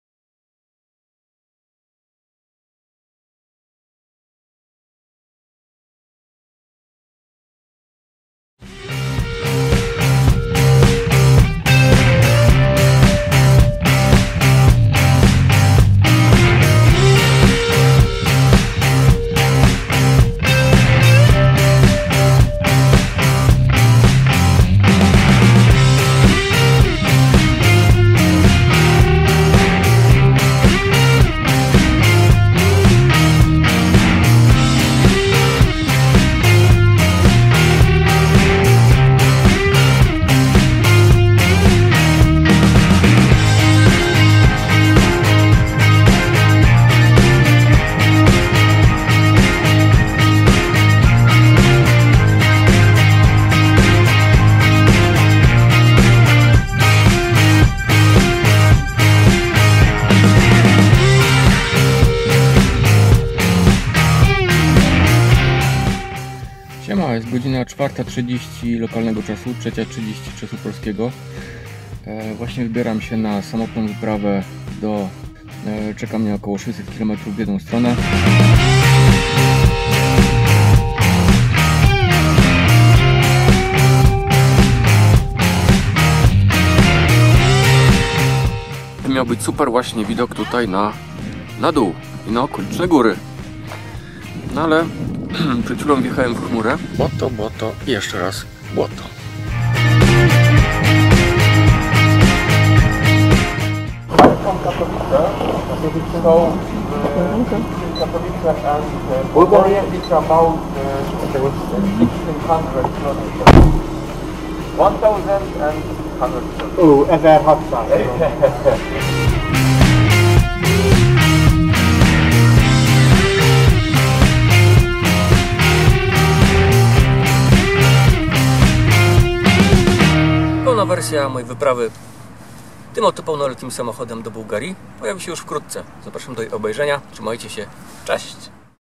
Bułgaria - Trailer
Zapowiedź relacji z wyjazdu do Bułgarii w roku 2020.